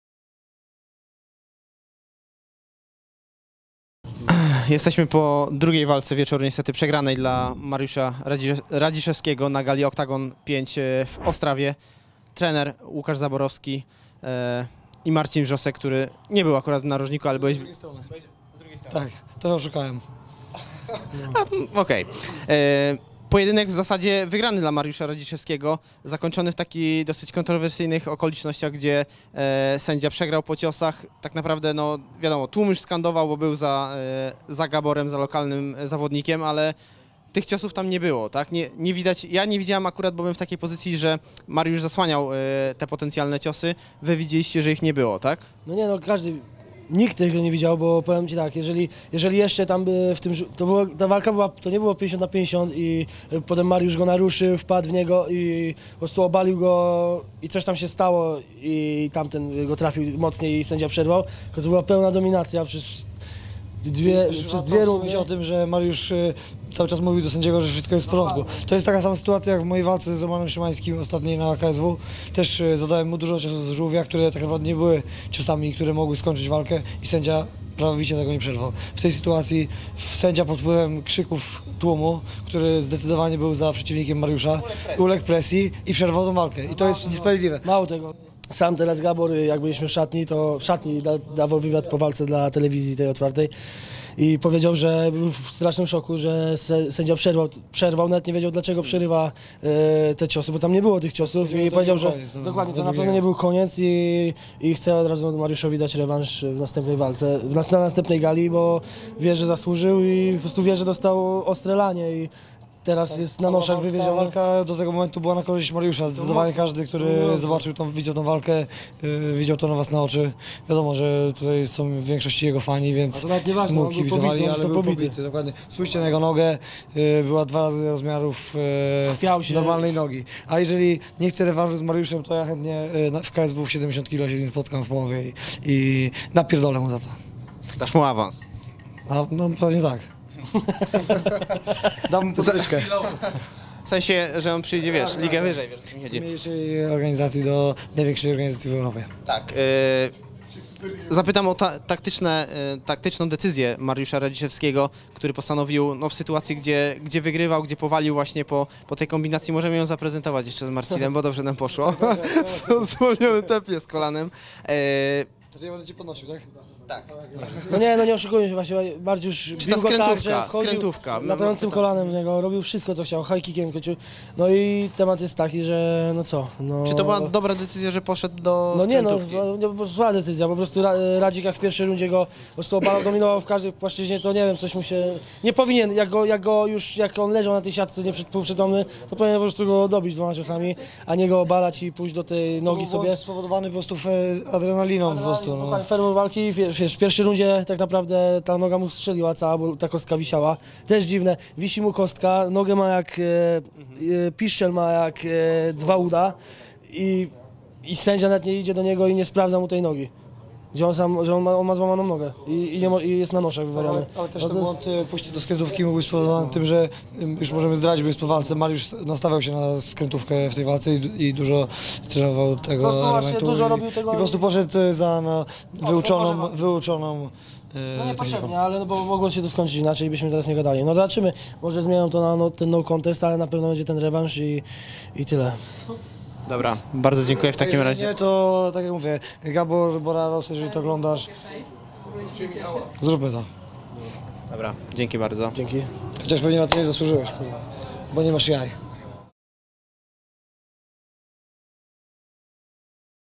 Marcin Wrzosek chętny pomścić Radziszewskiego: "Napierdolę Gaborowi w 70 kg."
Trener Łukasz Zaborowski i Marcin Wrzosek w podsumowaniu walki Mariusza Radziszewskiego w czeskim Oktagon 5, gdzie "Zombie" rzucił wyzwanie w kierunku niesłusznego ich zdaniem zwycięzcy - Gabora Borarosa.